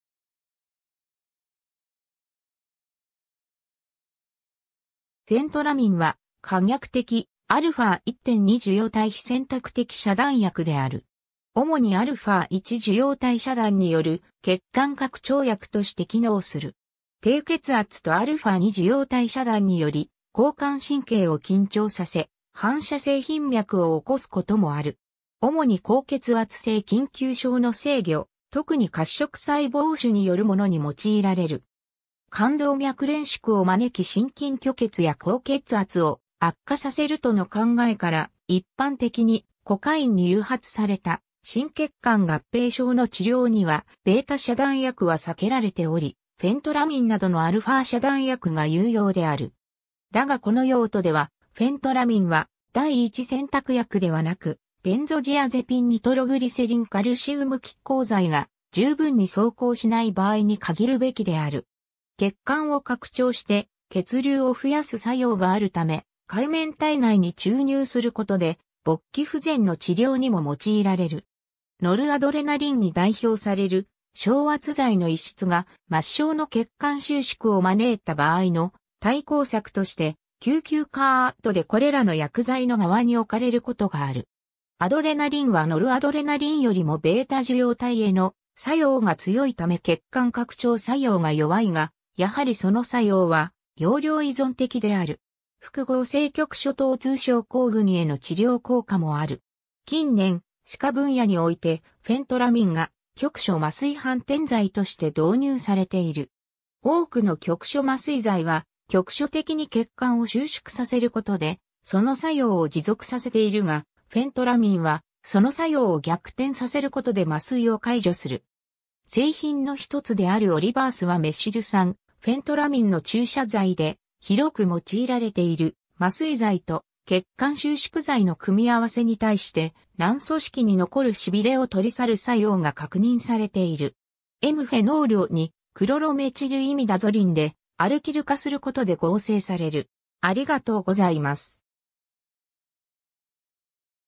フェントラミン
フェントラミン(Phentolamine、レジチン(Regitine))は可逆的α1･2受容体非選択的遮断薬である。
 主にα1受容体遮断による血管拡張薬として機能する。
 低血圧とα2受容体遮断により交感神経を緊張させ、反射性頻脈を起こすこともある。
 主に高血圧性緊急症の制御、特に褐色細胞腫によるものに用いられる。
 冠動脈攣縮を招き心筋虚血や高血圧を悪化させるとの考えから、一般的に、コカインに誘発された心血管合併症の治療にはβ遮断薬は避けられており、フェントラミンなどのα遮断薬が有用である。
だがこの用途ではフェントラミンは第一選択薬ではなく、ベンゾジアゼピン・ニトログリセリン・カルシウム拮抗剤が十分に奏効しない場合に限るべきである。
 血管を拡張して血流を増やす作用があるため、海綿体内に注入することで勃起不全の治療にも用いられる。
 ノルアドレナリンに代表される昇圧剤の溢出が末梢の血管収縮を招いた場合の対抗策として、救急カートでこれらの薬剤の側に置かれることがある。
アドレナリンはノルアドレナリンよりもβ受容体への作用が強いため血管拡張作用が弱いが、やはりその作用は用量依存的である。
 複合性局所疼痛症候群への治療効果もある。
 近年、歯科分野において、フェントラミンが局所麻酔反転剤として導入されている。
多くの局所麻酔剤は局所的に血管を収縮させることでその作用を持続させているが、フェントラミンはその作用を逆転させることで麻酔を解除する。
製品の一つである OraVerse はメシル酸フェントラミンの注射剤で、広く用いられている麻酔剤と血管収縮剤の組み合わせに対して、軟組織に残る痺れを取り去る作用が確認されている。
 m-(p-トルイジノ)フェノールを2-クロロメチルイミダゾリンでアルキル化することで合成される。